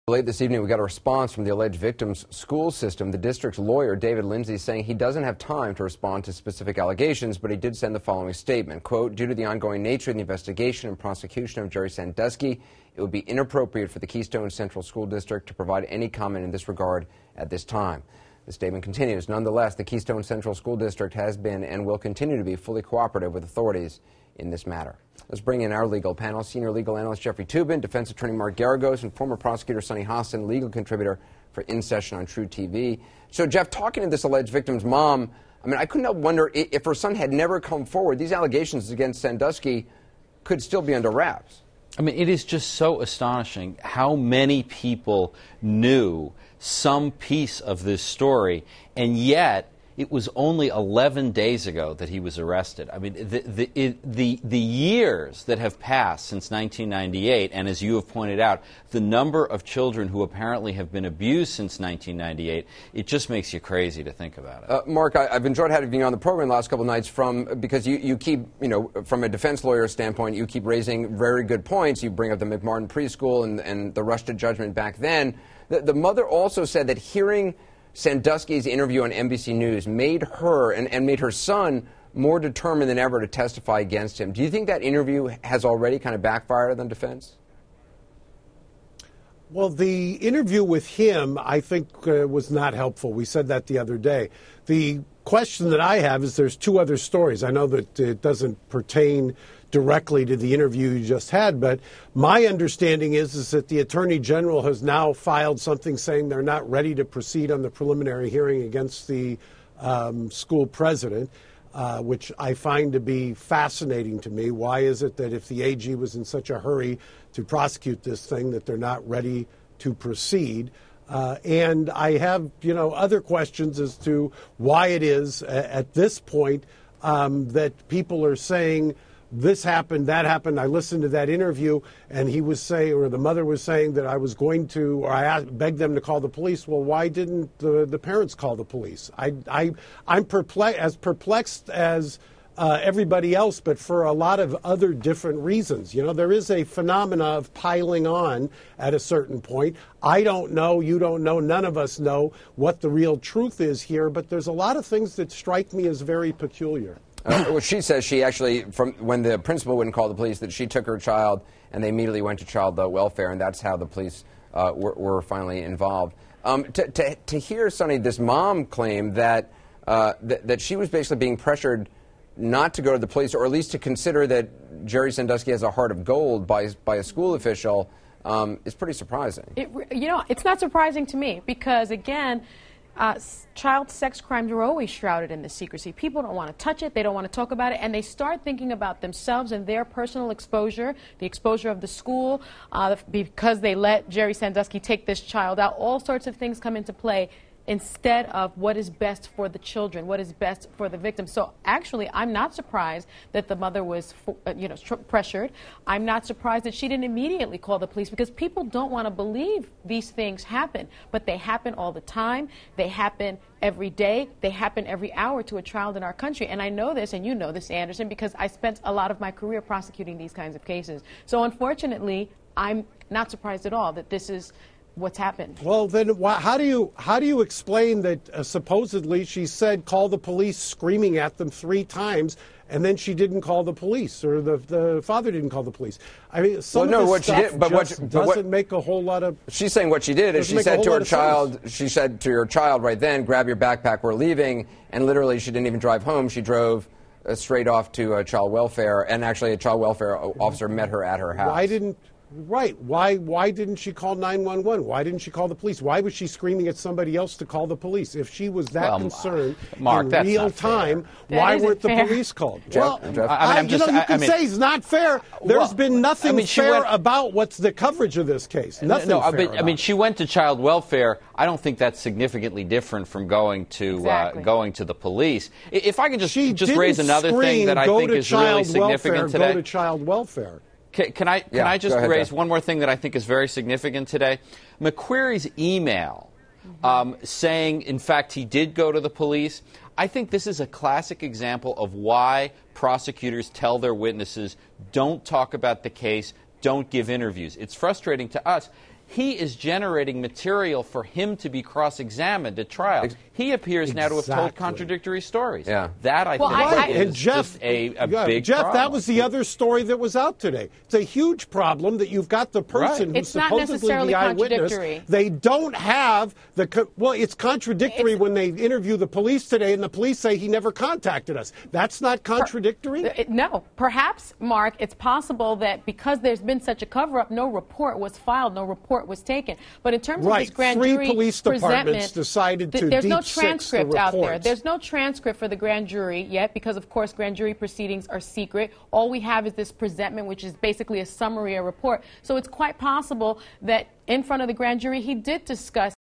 Penn State legal debate
A panel of legal experts discuss the evidence in the investigation and the allegations against Jerry Sandusky.